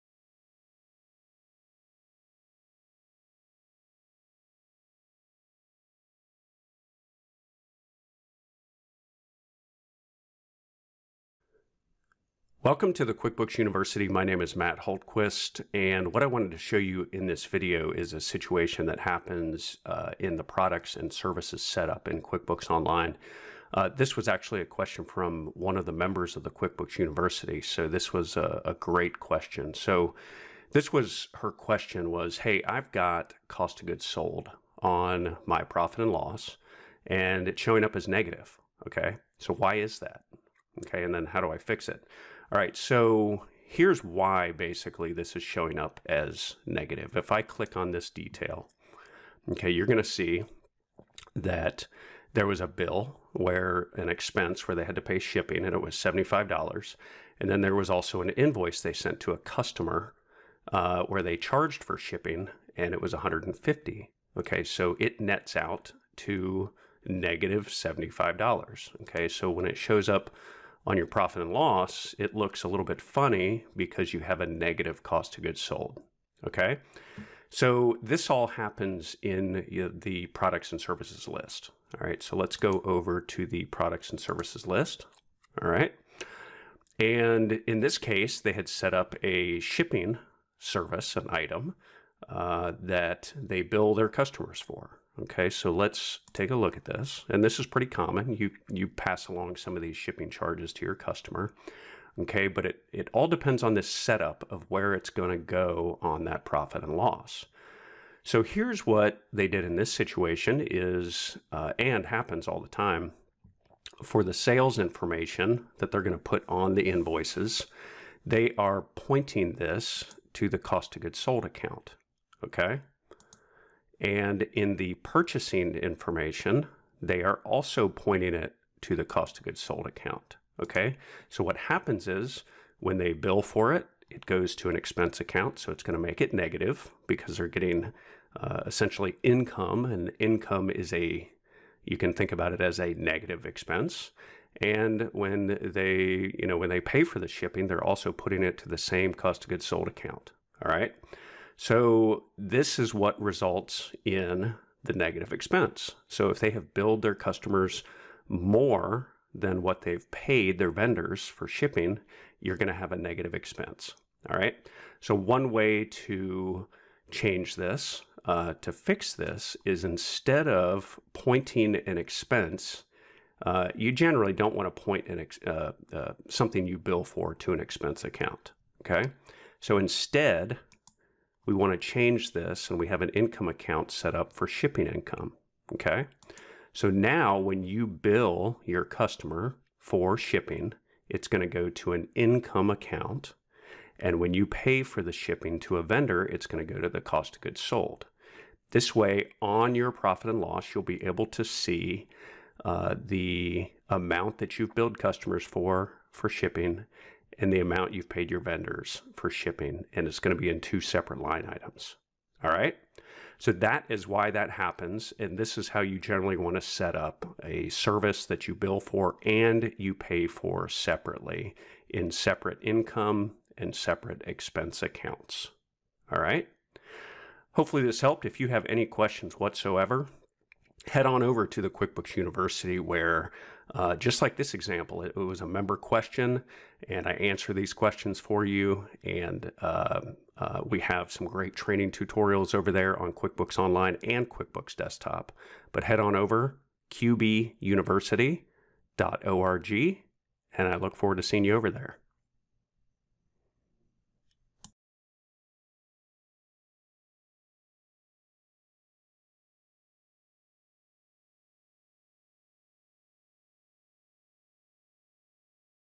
How to Fix Negative Expenses in Quickbooks Online
- This QuickBooks Online Tutorial was a GREAT question from a Member.  What to do when an expense shows up negative on your P&L.  In this case, which happens all the time, the Product & Service Item was set-up incorrectly...here's how to set-up your Product & Service Item the RIGHT way!

For more free videos, full training series plus personal support for all your QuickBooks questions, please